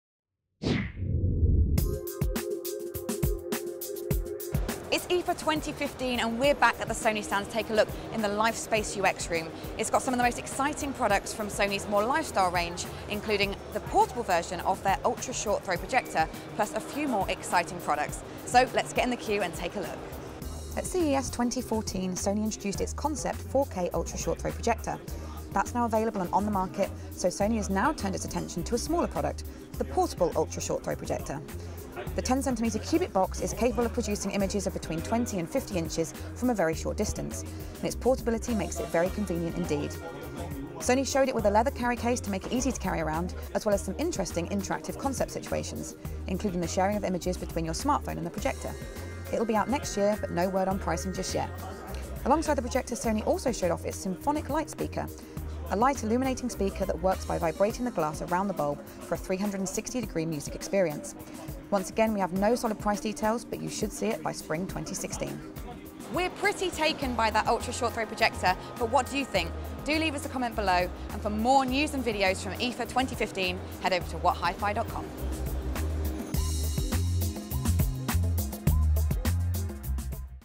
Sony Portable Ultra Short Throw Projector - first look
One of the most intriguing things we saw at IFA 2015 was Sony's new Ultra Short Throw Projector, on show in Sony's Life Space UK lifestyle innovations room. We popped in to have a look.